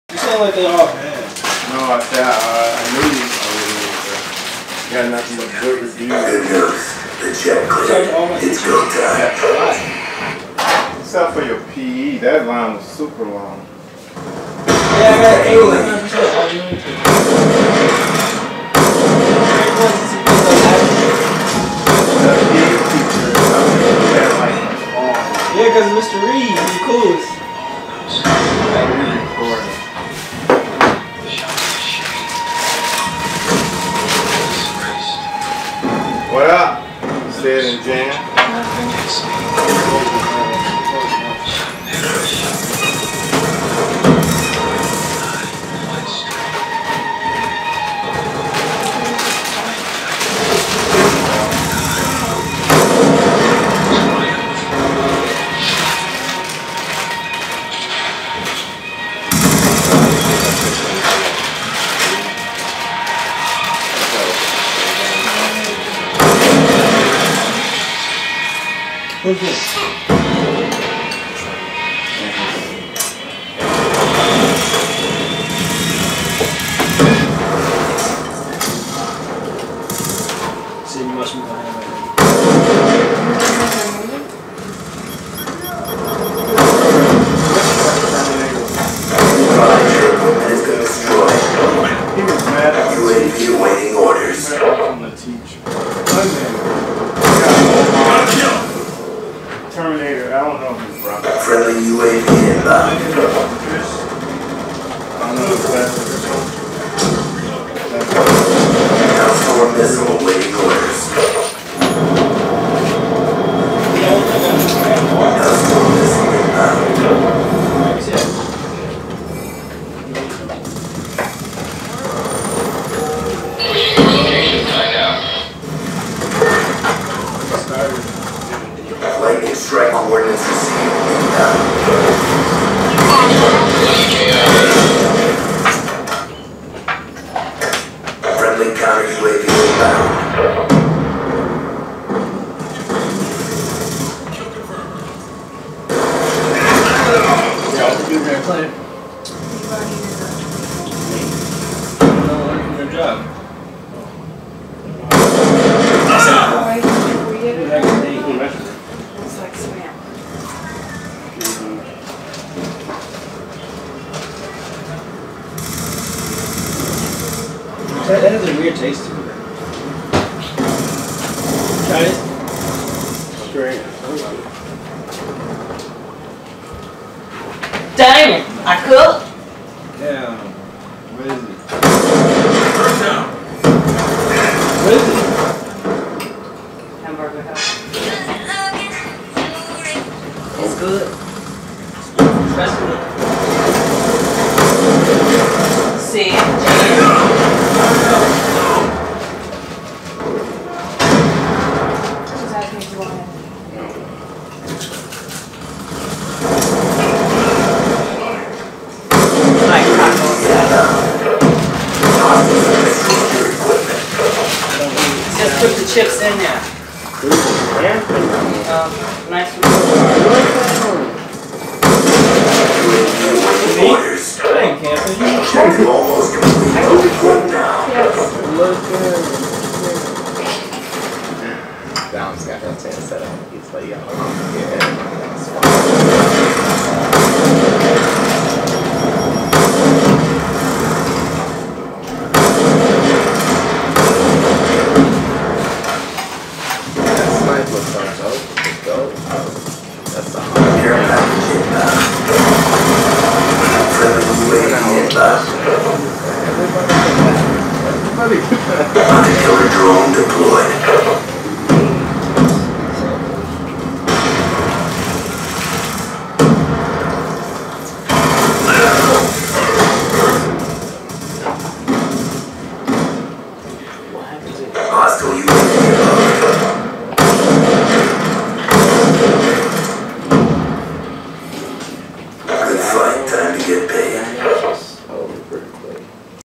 ENV3LOPE Gamplay
2 player teamwork from team Extraction with ENV3LOPE and RD Katrell 22. Holding down the map. Streaks!!!